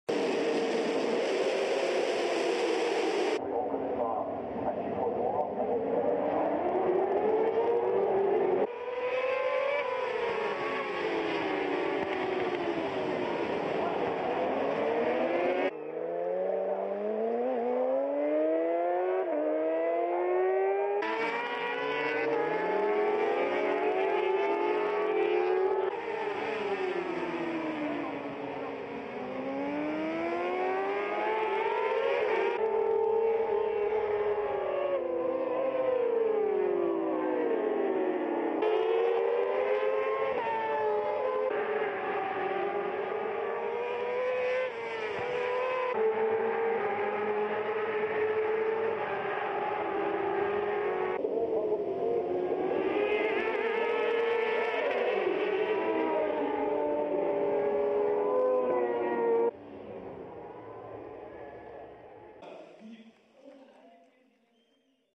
STK600 Race - Magny Cours 2011
Joy for Van der Mark as Metcher moves ahead